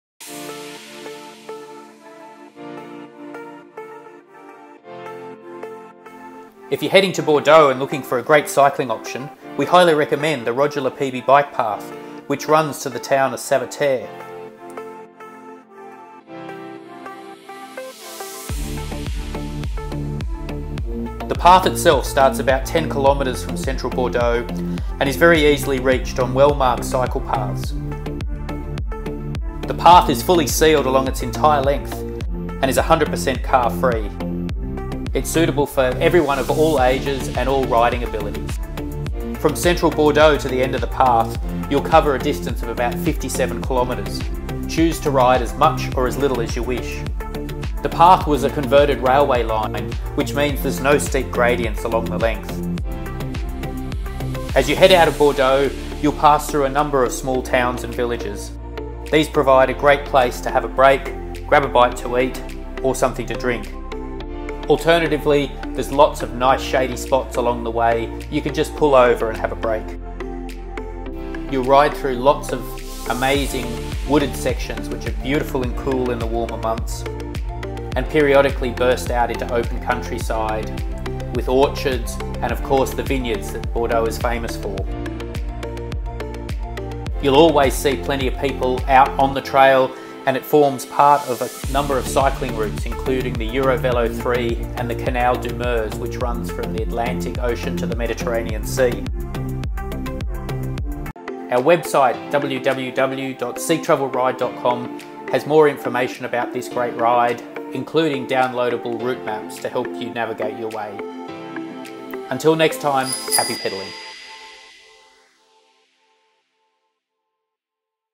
Roger Lapébie cycle path Bordeaux
If you are planning a trip to Bordeaux and looking for some ideas on what to do when you get there, we highly recommend the Roger Lapébie bike path.
The path is suitable for cyclists of all abilities and is totally car-free making it great for families.

For more information head to our Bordeaux pages on our